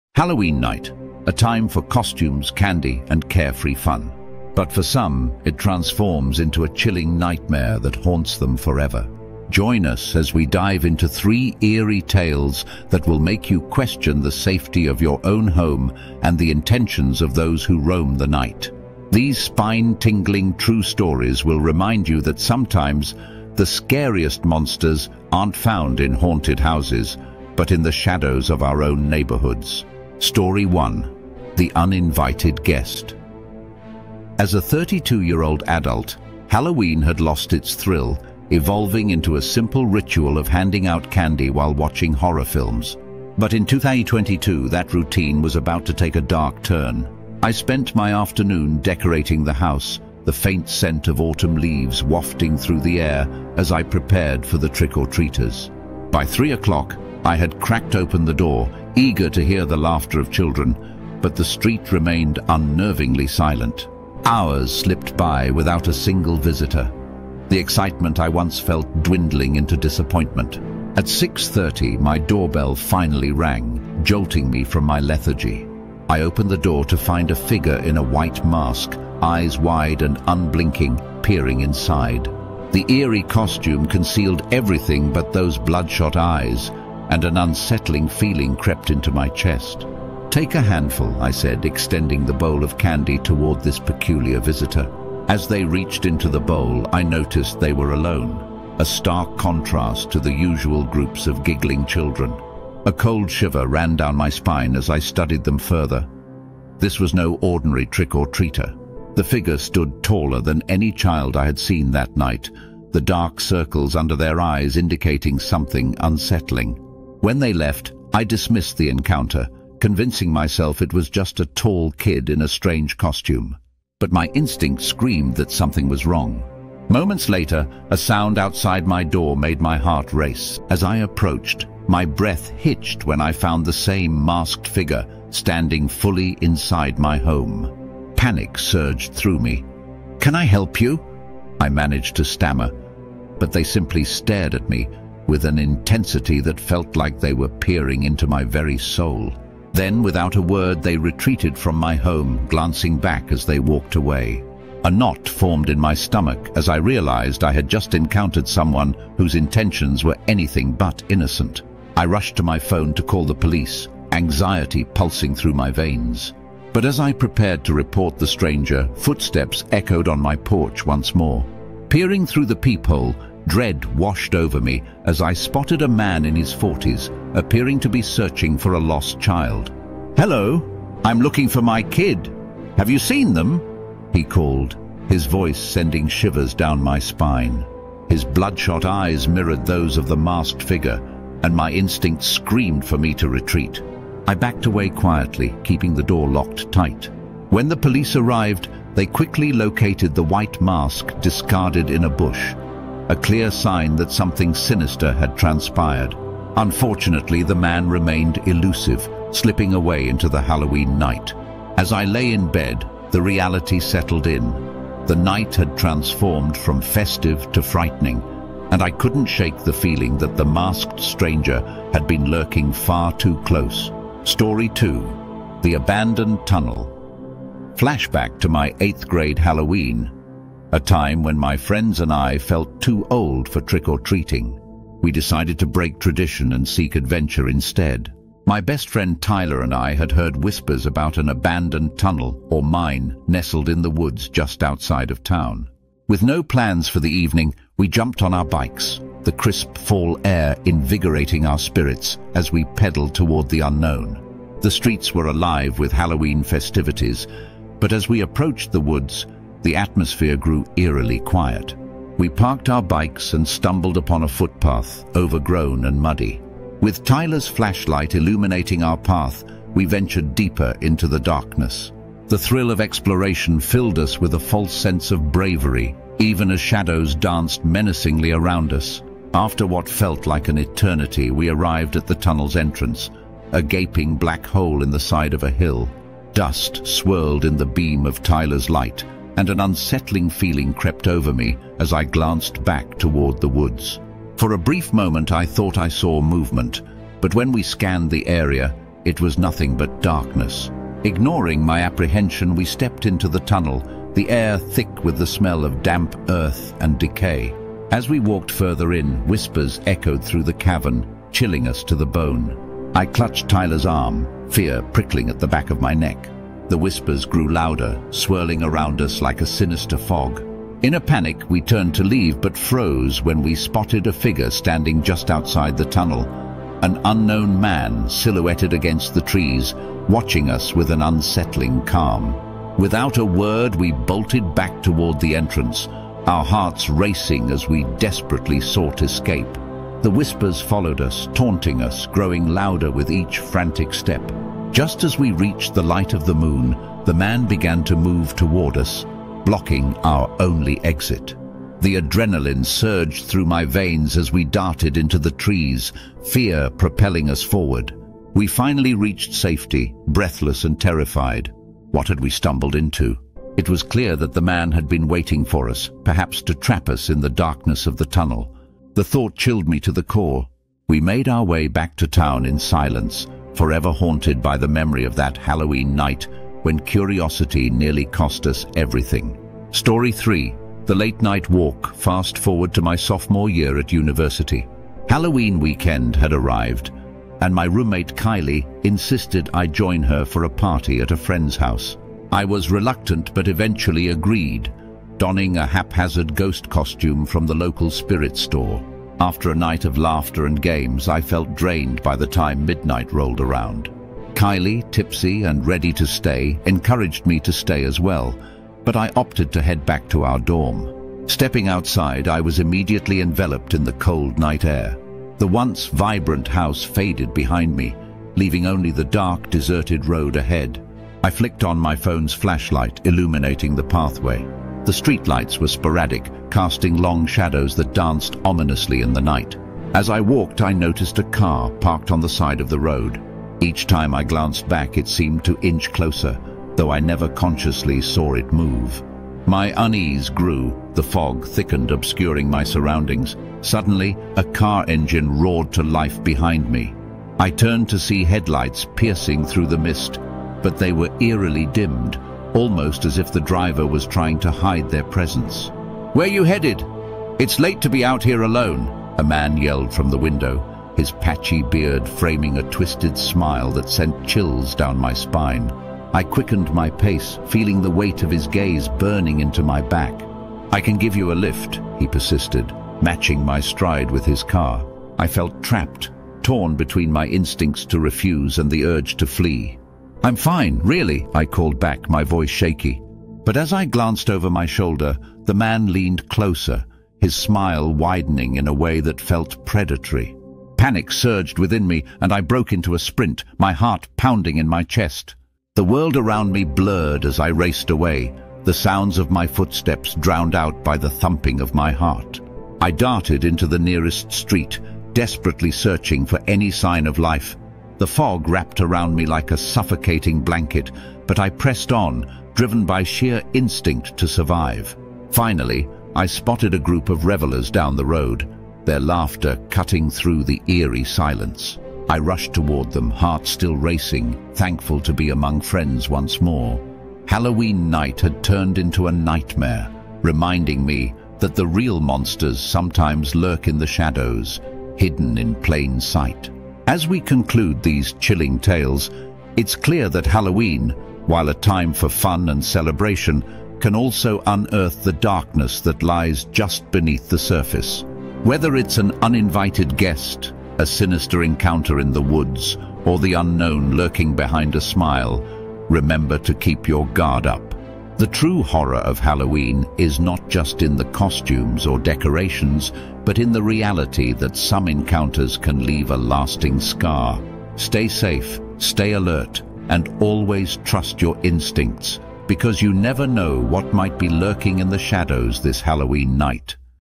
Real-Life Halloween Horrors 3 Creepy Stories You Won't Believe
🎃 Welcome to our Halloween Special! 🎃 Get ready to hear three true and terrifying stories that happened on Halloween night. These aren't your typical ghost tales—these are real-life accounts that will send shivers down your spine and make you think twice about venturing out alone on Halloween.

In this video, we dive into some bone-chilling experiences that people have lived to tell. From strange encounters to unexplained happenings, each story is more unnerving than the last. So, grab some popcorn, turn off the lights, and let yourself be drawn into the mysterious world of Halloween night horrors. 🌕👻

✨ Stories Included:

A late-night trick-or-treater encounter that went horribly wrong...
The eerie prank that turned into something truly sinister...
An unexpected guest at a Halloween party who left everyone in fear...

If you’ve had your own creepy Halloween experiences, share them in the comments below—we’d love to hear them!